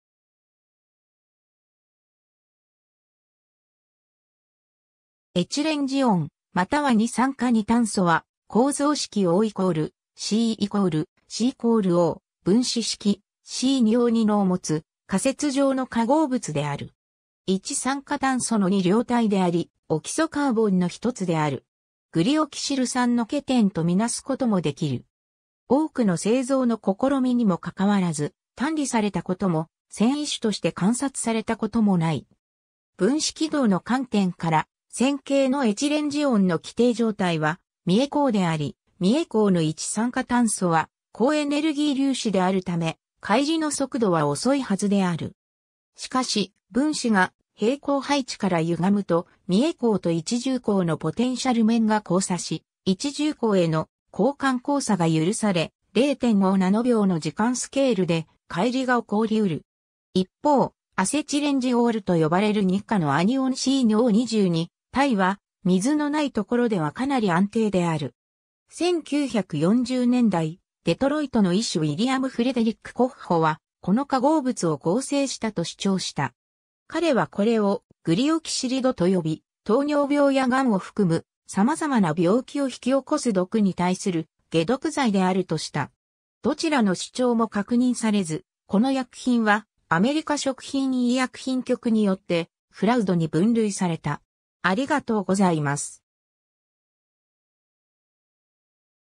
エチレンジオン
エチレンジオン(Ethylene dione)または二酸化二炭素(Dicarbon dioxide)は、構造式 O=C=C=O、分子式 C2O2 のを持つ仮説上の化合物である。
一酸化炭素の二量体であり、オキソカーボンの1つである。
グリオキシル酸(OHCCOOH)のケテンと見なすこともできる。
多くの製造の試みにも関わらず、単離されたことも遷移種として観察されたこともない。
 分子軌道の観点から、線形のエチレンジオンの基底状態は三重項であり、三重項の一酸化炭素は高エネルギー粒子であるため、解離の速度は遅いはずである。
しかし、分子が平衡配置から歪むと、三重項と一重項のポテンシャル面が交差し、一重項への項間交差が許され、0.5ナノ秒の時間スケールで解離が起こりうる。
 一方、アセチレンジオールと呼ばれる二価のアニオンC2O22-は、水のないところではかなり安定である。
 1940年代、デトロイトの医師ウィリアム・フレデリック・コッホは、この化合物を合成したと主張した。
彼はこれをグリオキシリド(glyoxylide)と呼び、糖尿病や癌を含む様々な病気を引き起こす毒に対する解毒剤であるとした。
どちらの主張も確認されず、この薬品は、アメリカ食品医薬品局によって、"fraud"（詐欺）に分類された。